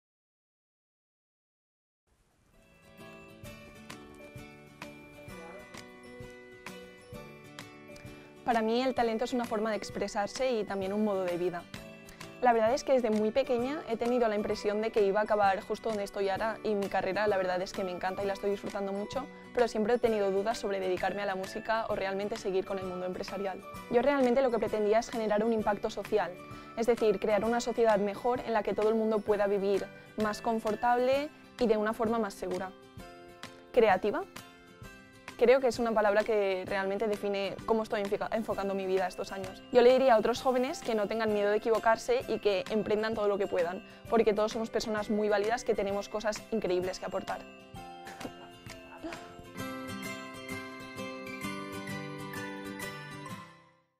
YoCreoTalento acompañamos el talento innovador
Acompañamos el talento innovador a través de premios como 'UPF Emprèn Idea', que en 2016 recibió el proyecto 'Compiso'' de Mar Villora.